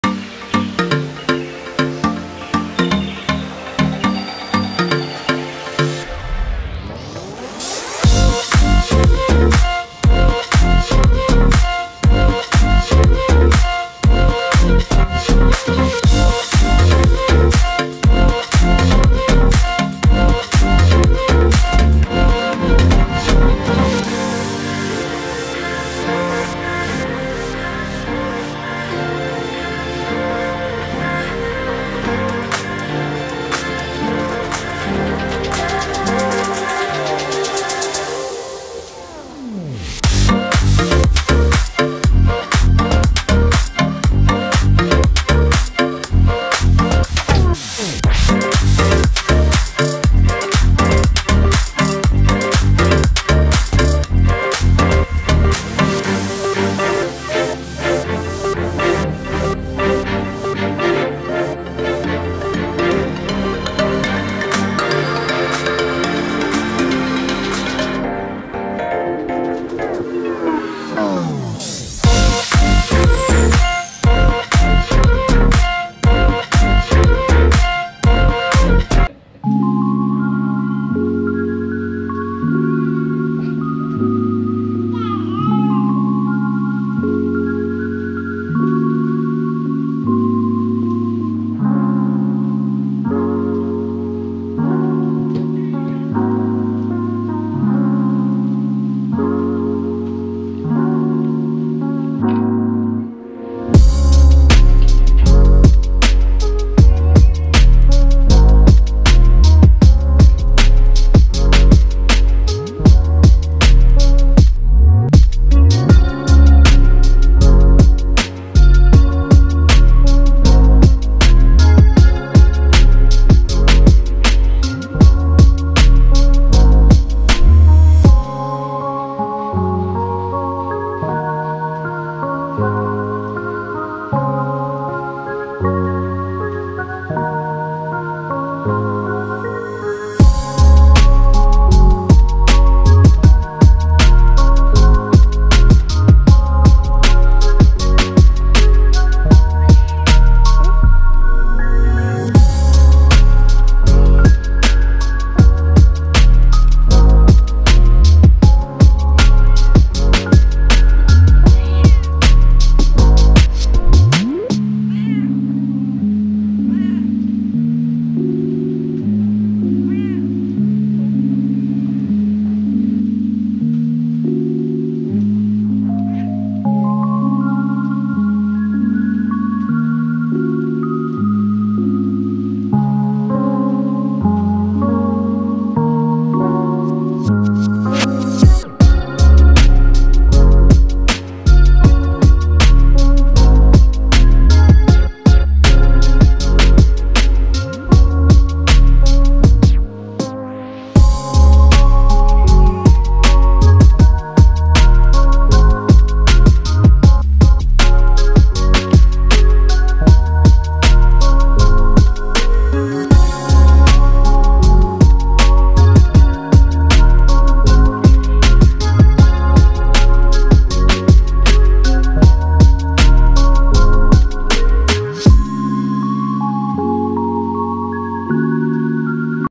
Random Phone Video Compilation #1 (Mostly Cats)
Getting my old files sorted. Making some compilation for slices of life before losing the videos.
Stray cats, cat cafe cats and my own cats